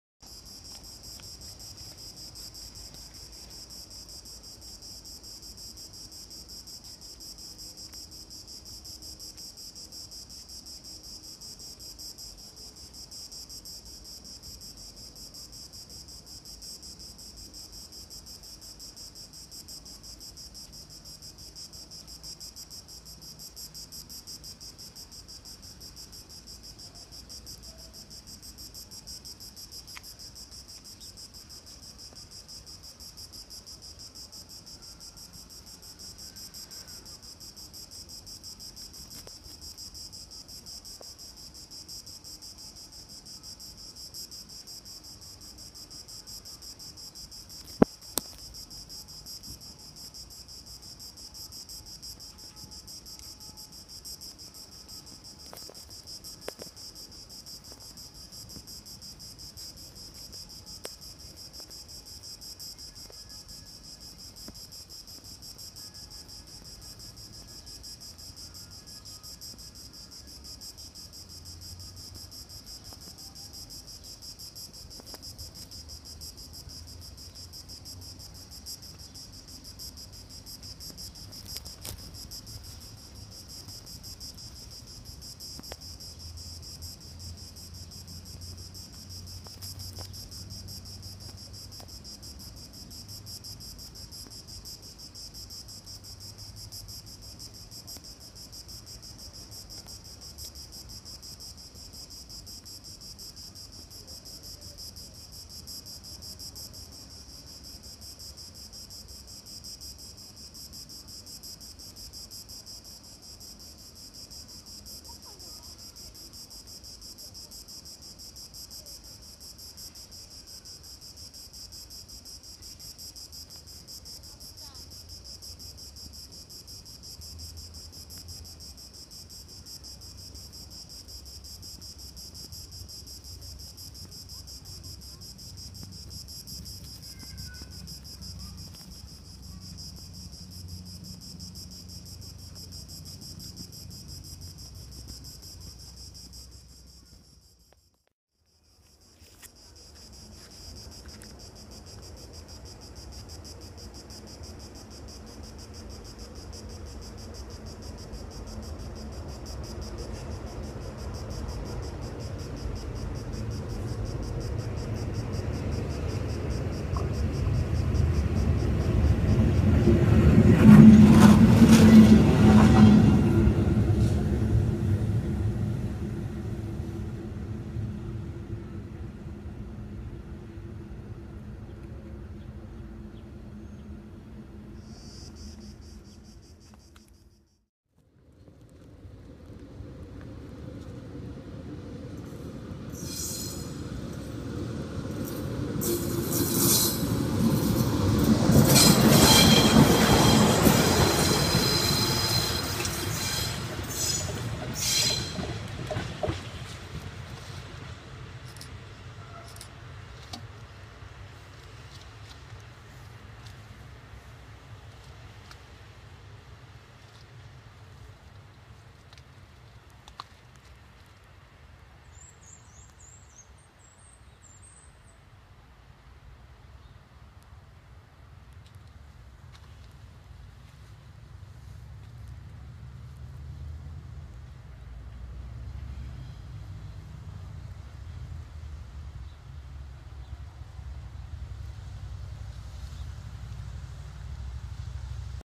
E.652-026 ARRIVO E RIPARTENZA STAZIONE DI SAN VINCENZO
E.652-026 --- O.M.R. CERVIGNANO --- I Serie.
Proveniente da Piombino e diretto a Livorno, questo LIS (locomotore isolato) espletato dalla E.652-026 (ITIN-Ansaldo, 1993) in livrea XMPR, il quale viene prontamente messo in "buca" sul terzo binario della stazione di San Vincenzo per lasciare la precedenza all'EXP 14164 pellegrini da Catania, con destinazione Lourdes. Nel video, in tre atti, si nota la macchina procedere a rilento (con un fanale di testa non funzionante) verso il deviatoio ad una velocità di circa 30 Km/h. Dopo aver lasciato la precedenza al treno pellegrini e ad un merci d'acciaio trainato dalla E.405-035, diretto invece a Sud, la E.652-026 riprende la sua corsa uscendo dalla stazione e rimettendosi in corretto tracciato.
(Video di: Luca Cesana).